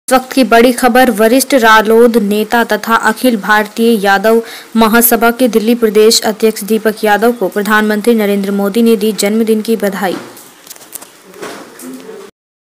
अखिलभारतिययादवमहासभा के दिल्ली अध्यक्ष दीपक यादव को प्रधानमंत्री नरेन्द्र मोदी ने दी जन्मदिन की बधाई
बडी खबर
वरिष्ठ रालोद नेता तथा अखिल भारतिय यादव महासभा के दिल्ली प्रदेश अध्यक्ष दीपक यादव को प्रधानमंत्री नरेन्द्र मोदी ने दी जन्मदिन की बधाई !  जी न्यूज इंडिया की रिर्पोट